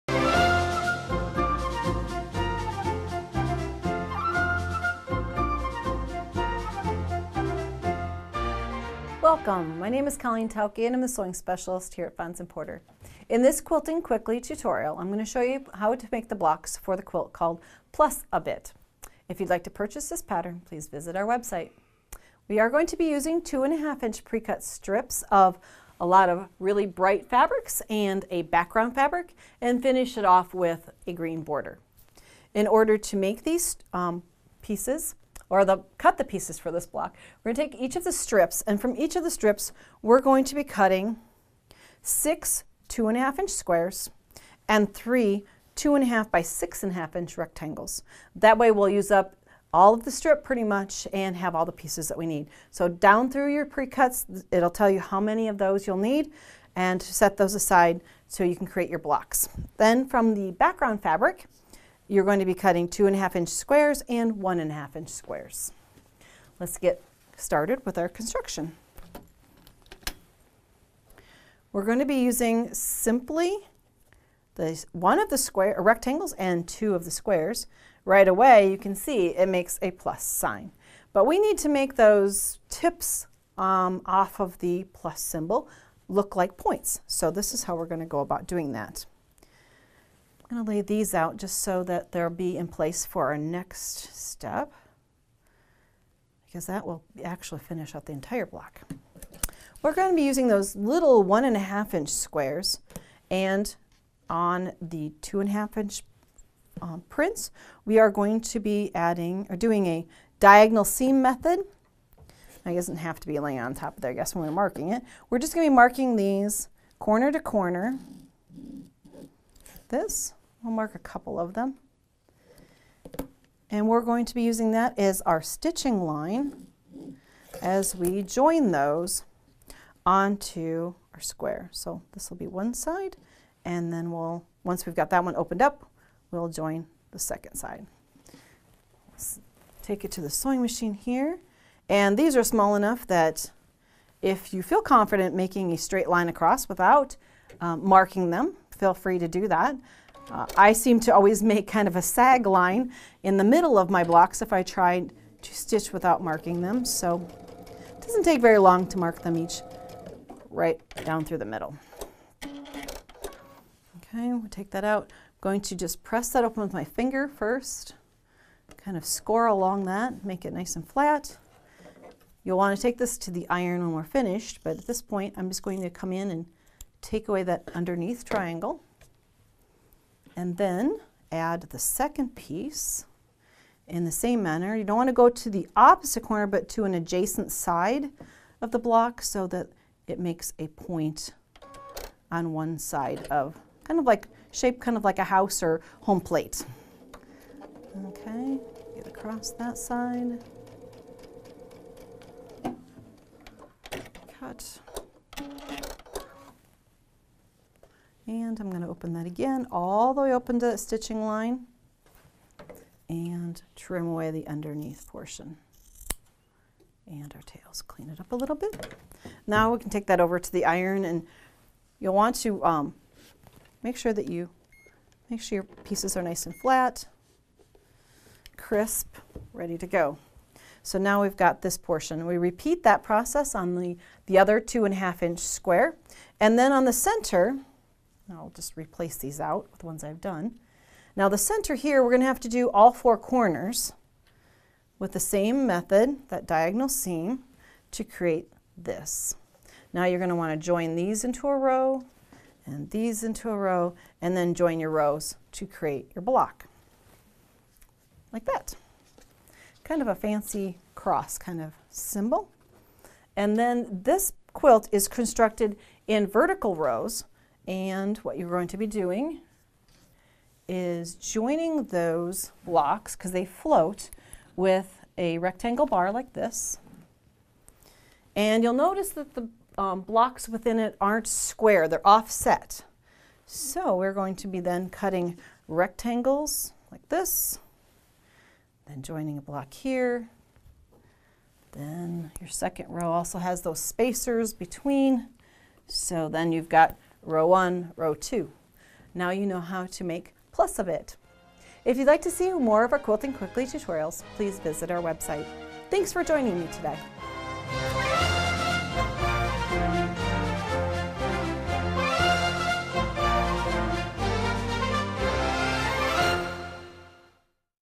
Quilting Quickly - Easy Plus A Bit Quilt
Calling all fans of colorful quilts! This easy bed-size quilt pattern uses 2½” pre-cut strips and diagonal seam piecing in bright colors on a white background. This quilt would look great on a teen’s bed and goes with any paint color on the walls.